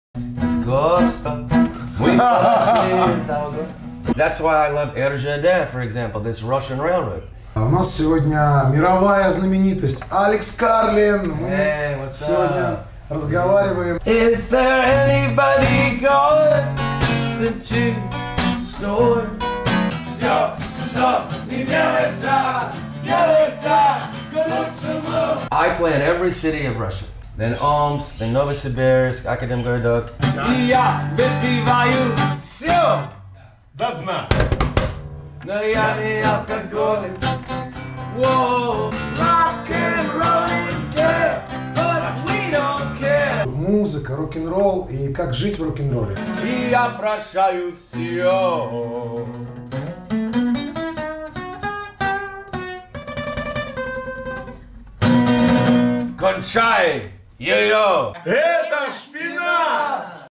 Alex Carlin интервью
Интервью с AlexCarlin специально для проекта «Музон.Ру»
Alex Carlin - уникальный рок-музыкант из США (Сан-Франциско, Калифорния), который занесен в книгу рекордов Гиннеса за самый продолжительный концерт в истории мировой рок-музыки - 32 часа!!!
Отдельное спасибо вкусному ресторану грузинской кухни Зандукели's Шпинат и всем его работникам за то что приютили и накормили нас!!!
Москва, Вознесенский переулок 14
Camera: @KasatkinProduction